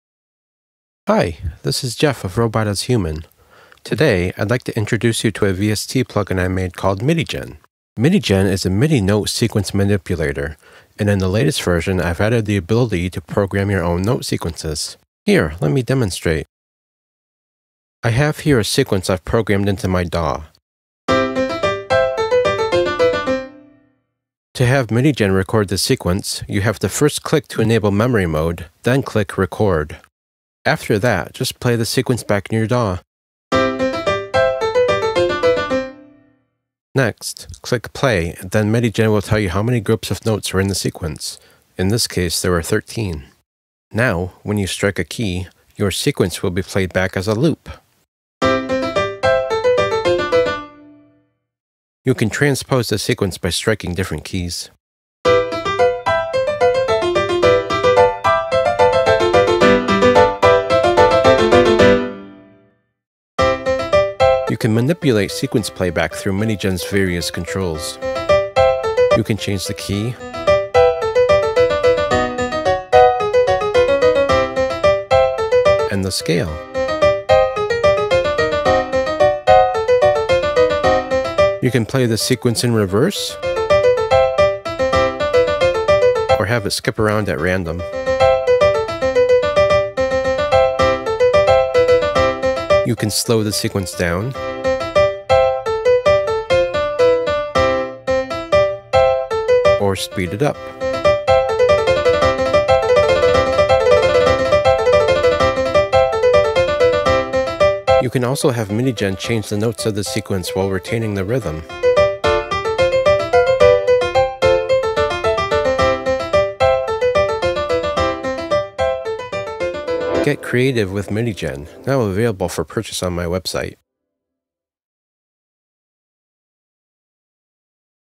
MIDIGen VST: A MIDI note sequence manipulator
MIDIGen is a VST plugin I made that can take a sequence of MIDI notes and manipulate many aspects of it. It can, in real-time, change the sequence's key, the scale, the pitch, the tempo, and more. This video demonstrates how to program a custom note sequence with the plugin, which is a new feature of version 1.1.0.